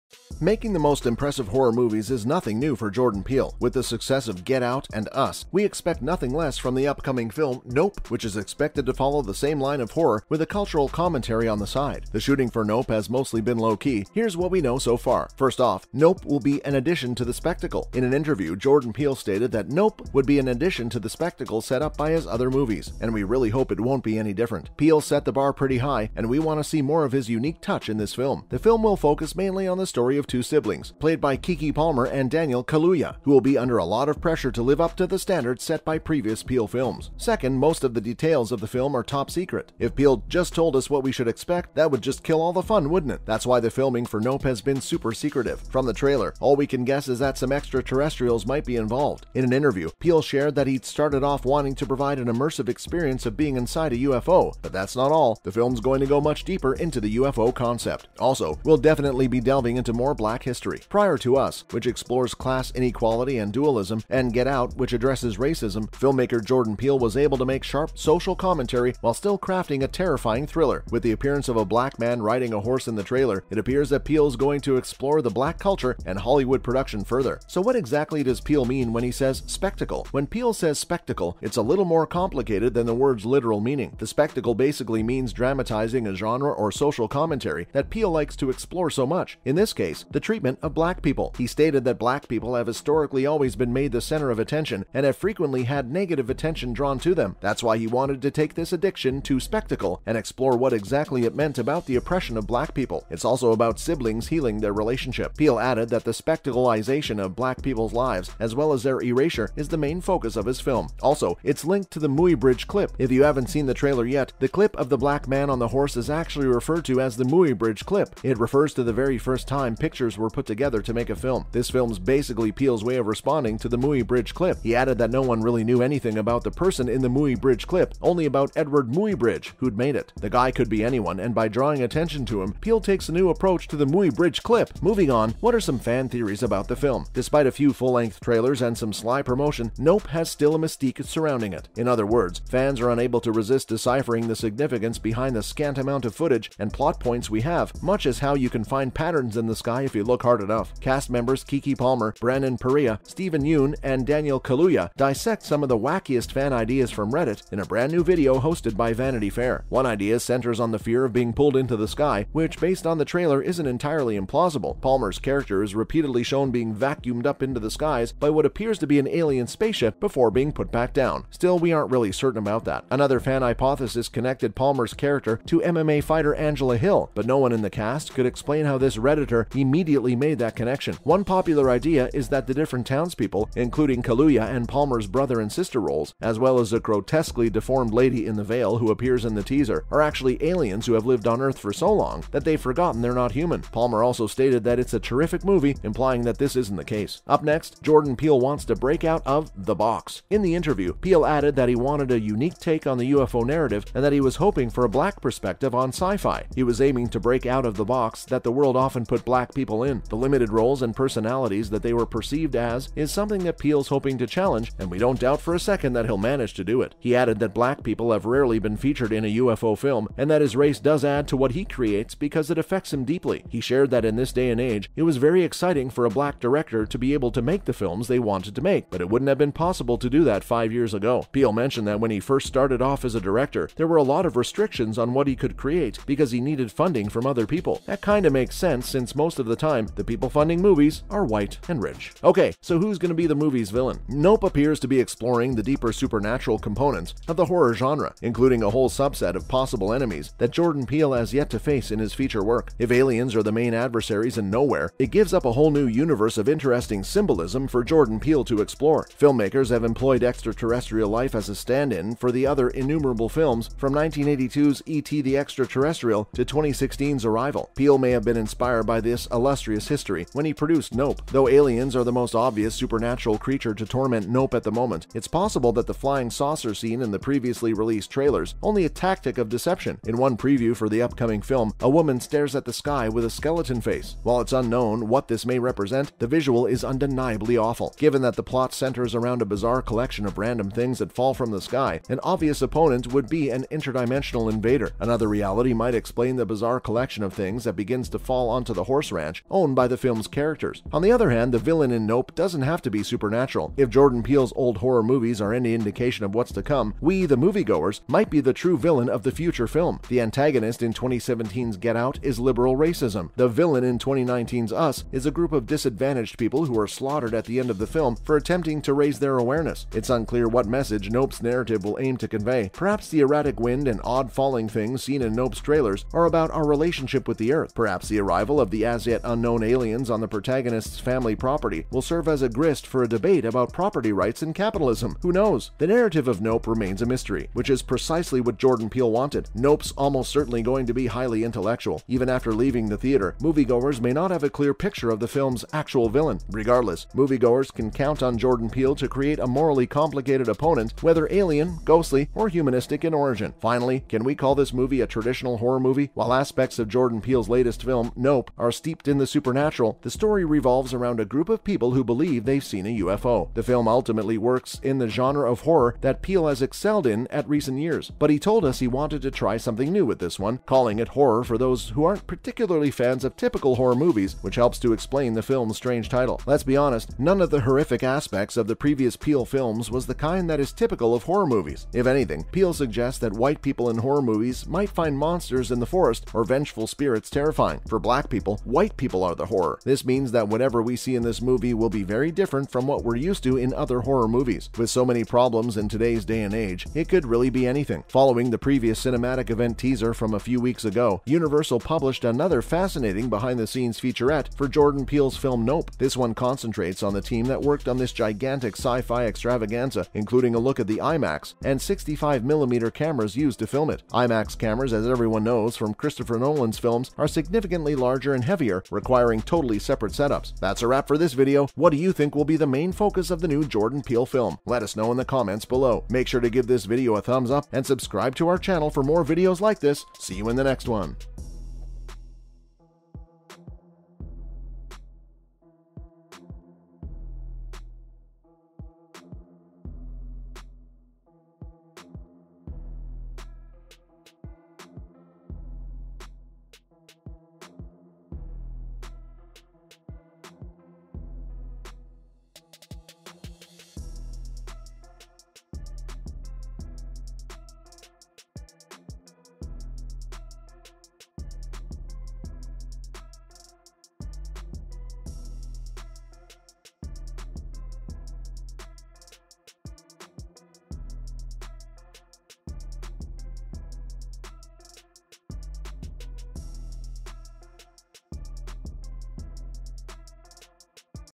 'Nope' NEW Details Have Been REVEALED..
Welcome back to Fantasy Focus, today we are going to be talking about  nope’ movie new details revealed and why fans are so excited. So make sure you stick around to find out  nope’ movie new details revealed and why fans are so excited!